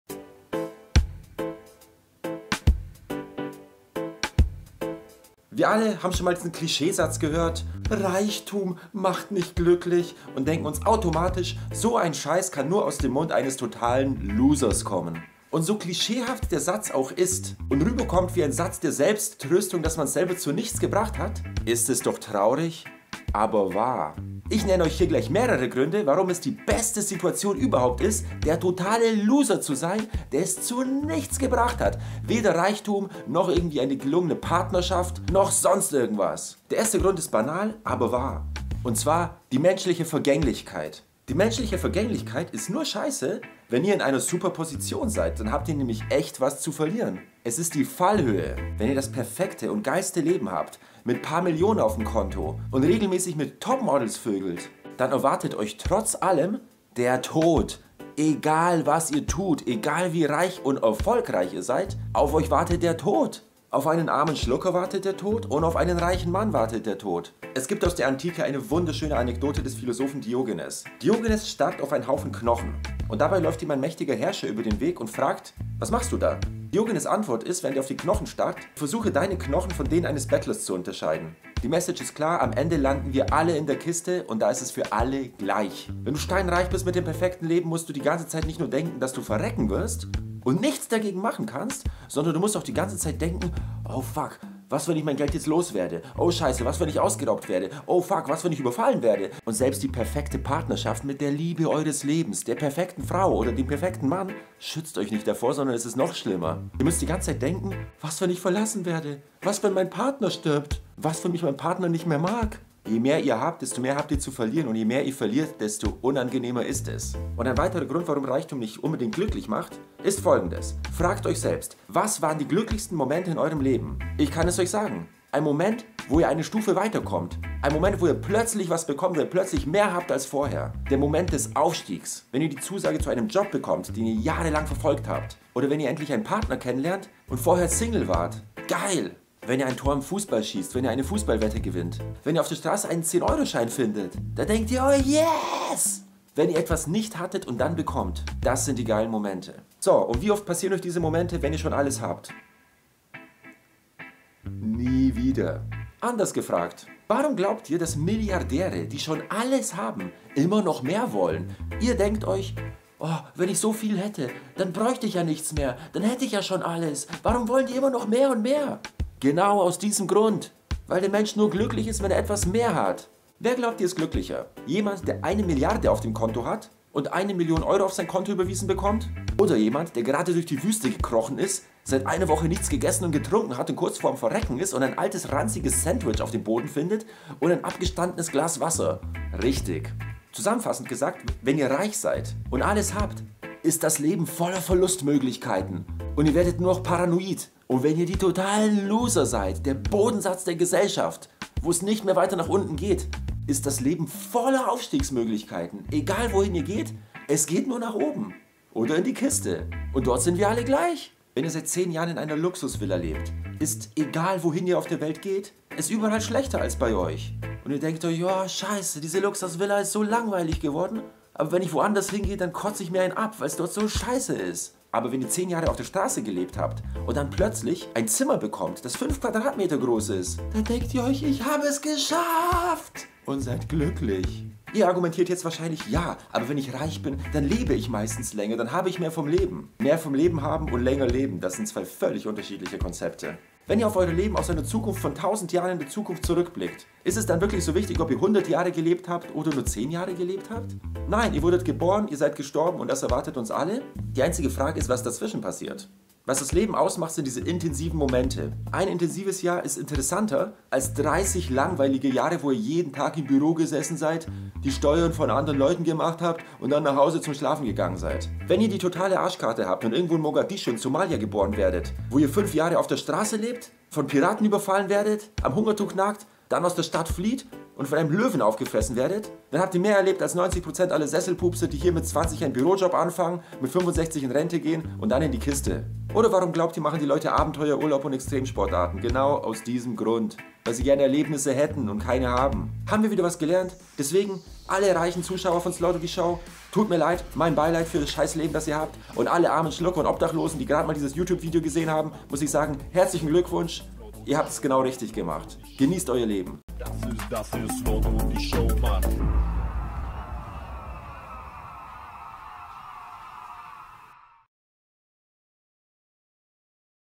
Tatsache: Loser sein ist besser als Reichtum!
Warum bringen sich so viele Reiche um, sterben früh oder haben Depressionen? Und warum werden so viele Loser alt und glücklich? Hier die Gründe, einfach erklärt! Choose your lifestyle wisely!
Die politisch unkorrekte Comedyshow mit einem Touch Philosophie und Lebenstipps, von und mit Jakub Slodowicz